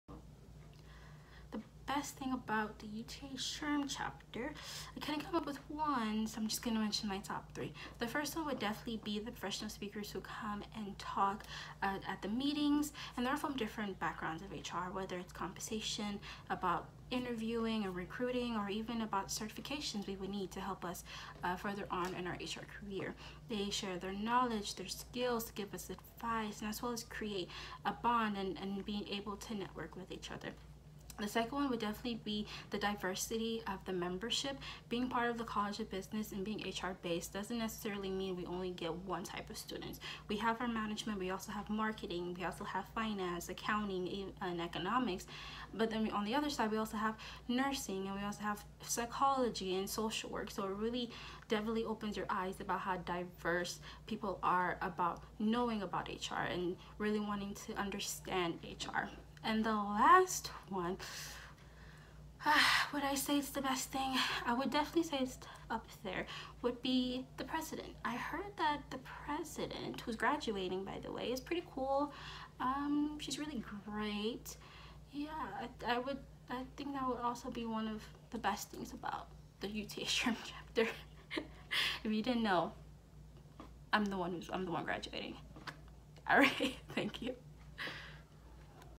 Tsedenya Gebreyesus on the Best Thing about UTA SHRM
This is number 6 in a short series.
Tsedenya Gebreyesus is a senior Management major in the UTA College of Business (soon to be BBA'20) and the President of UTA SHRM. We asked Syd what's the best thing about UTA SHRM and this is what she shared with us: